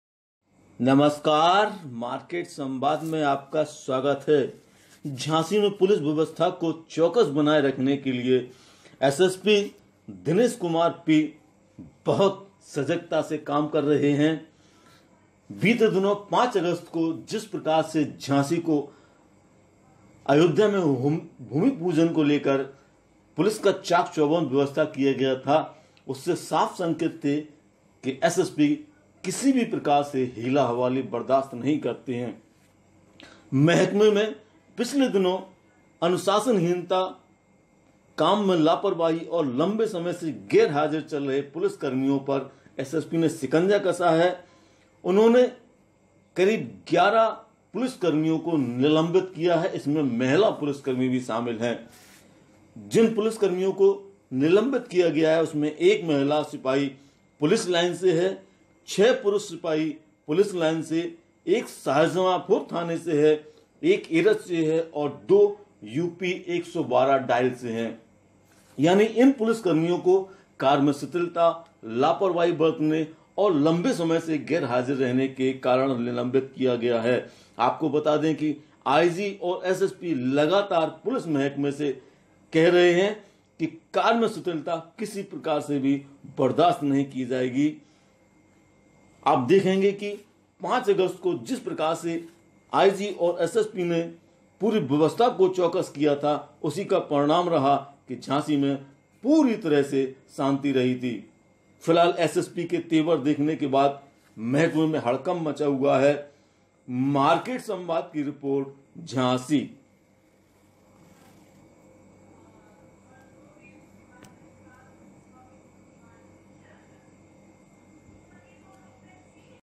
झाँसी एसएसपी ने क्यों निलबिंत किए पुलिस कर्मी ?
झांसी में अनुुशासन हीन लापरवाह पुलिसकर्मियों के खिलाफ एसएसपी ने सख्त रुख अपनाना शुरु कर दिया है। एसएसपी के रडार पर विभागीय कार्य में शिथिलता बरतने, समय पर कार्य न करने और लम्बे समय से अनुपस्थित रहने वाले पुलिसकर्मी हैं। इस तरह के पुलिस कर्मियों के खिलाफ एसएसपी ने निलम्बन की कार्यवाही की है। जिससे पुलिस महकमें हड़कम्प मचा हुआ है।
एसएसपी दिनेश कुमार पी को जिले के पुलिस महकमें लापरवाही की लगातार शिकायतें मिल रही थीं। एसएसपी ने इसे गम्भीरता से लिया। उन्होंने ऐसे पुलिसकर्मियों को चिहिंत करना शुरु कर दिया है जो काफी लम्बे समय से बिना अवकाश या फिर बिना अनुमति से गैरहाजिर चल रहे हैं या फिर ड्यूटी से एक माह से अधिक गैरहाजिर है।
एसएसपी ने ऐसे 11 पुरुष/महिला आरक्षियों के खिलाफ निलम्बन की कार्यवाही करते हुए विभागीय जांच शुरु कर दी है। जिन पुलिसकर्मियों को निलम्बित किया गया है उनमें 1 महिला सिपाही पुलिस लाइन से, 06 पुरुष सिपाही पुलिस लाइन से, 01 सिपाही शाहजहांपुर थाने से, 1 सिपाही थाना एरच से और दो सिपाही यूपी-112 से सम्बधित है। इसके अतिरिक्त थाना, शाखा/पुलिस लाइन से भी रिपोर्ट मांगी गई है जो